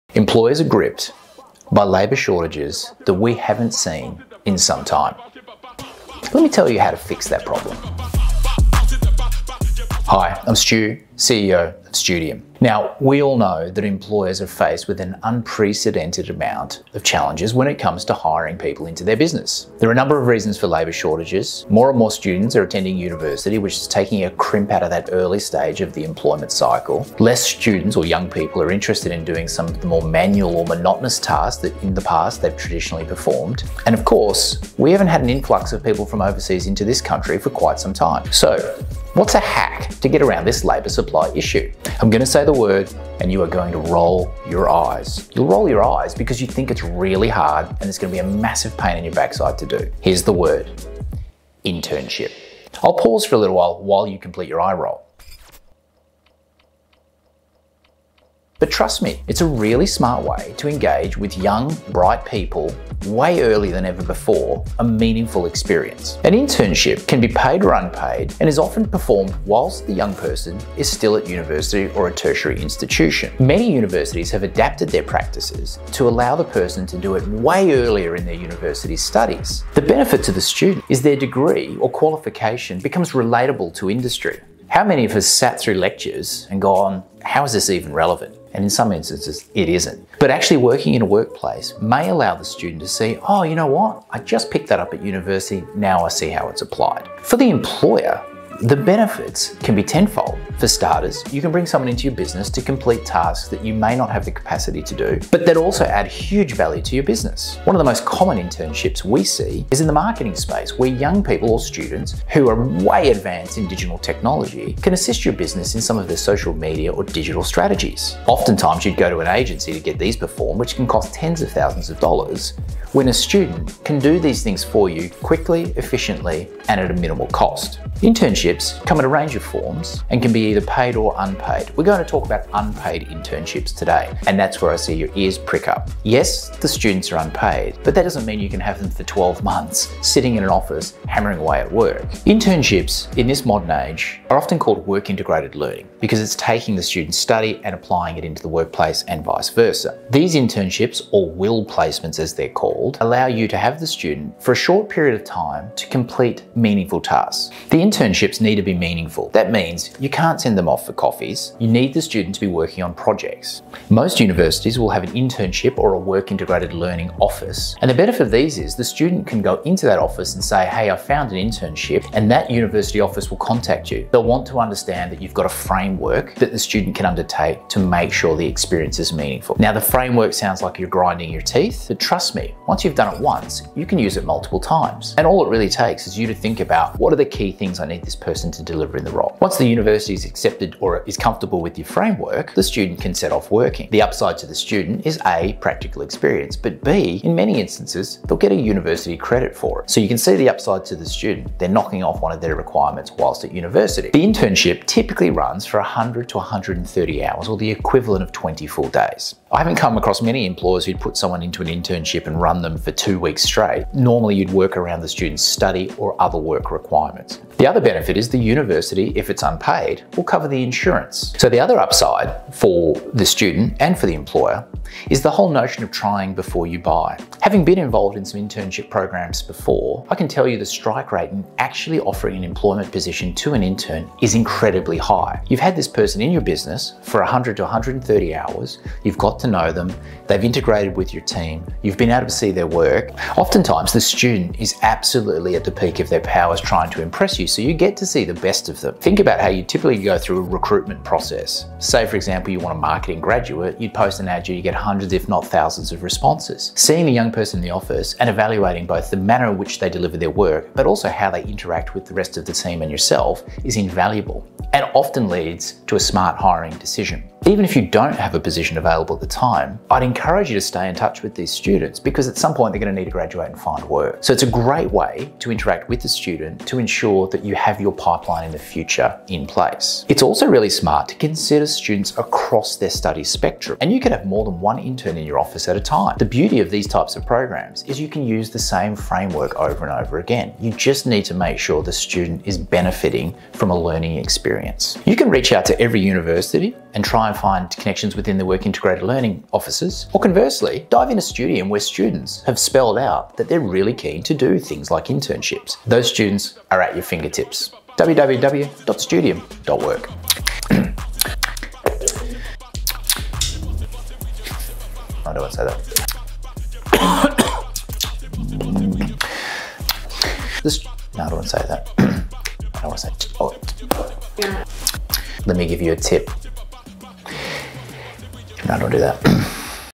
This Week in Hiring: The SOLUTION to Labour Shortages?
Employers are gripped by labour shortages we haven't seen in some time... Stu knows the solution!

For more information or to sign up to Studium head